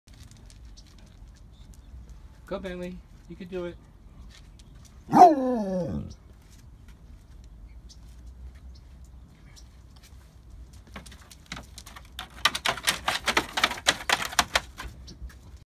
Basset walks up ramp
10 year old chubby basset with vision loss still manages going up ramp.